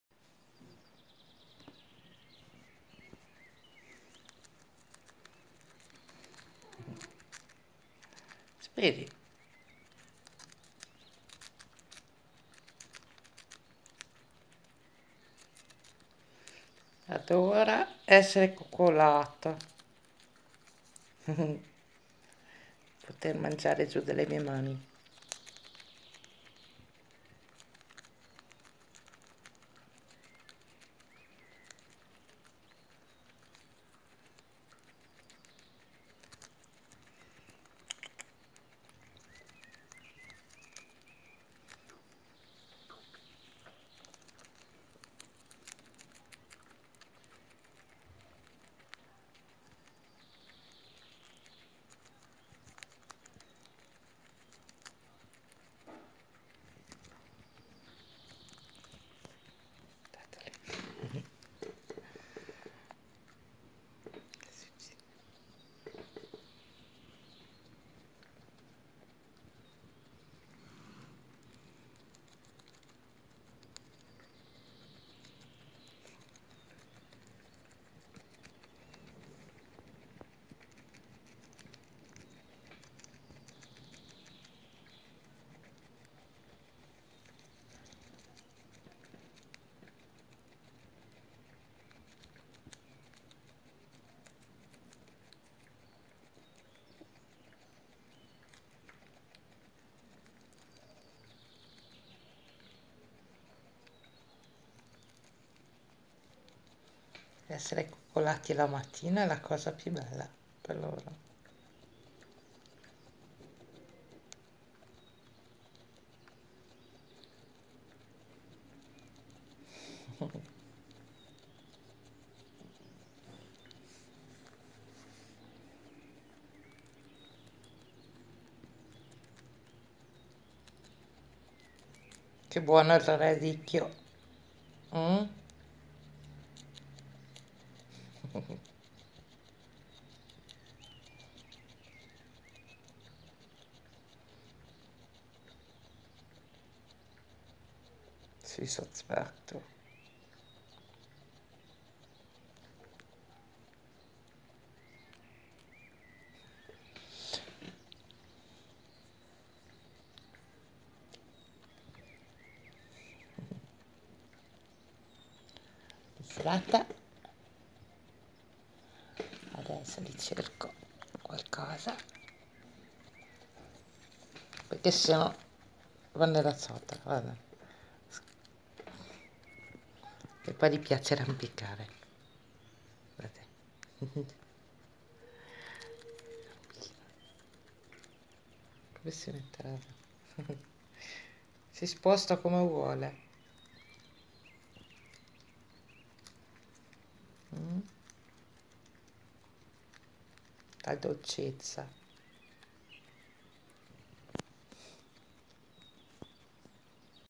Spidy adora essere imboccato
Oggi spidy è  5 giorni con noi! Ed già bello viziato! Come non si può voleri bene! Ha una dolcezza enorme! Un amore proprio ❤❤